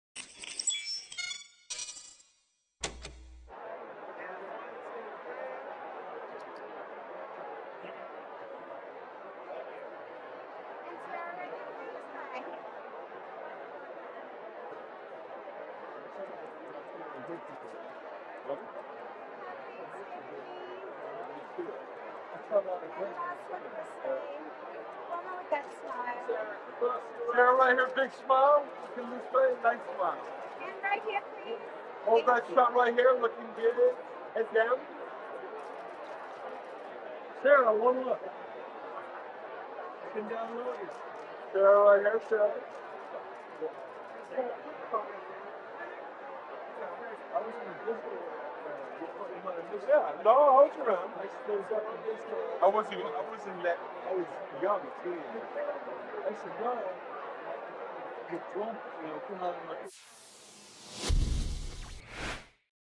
Sarah Bishop 2023 G'Day USA Arts Gala Blue Carpet Arrivals
Broll footage: Sarah Bishop on the blue carpet at the 20th anniversary of G'Day USA Arts Gala held at Skirball Center in Los Angeles, California USA on January 28, 2023.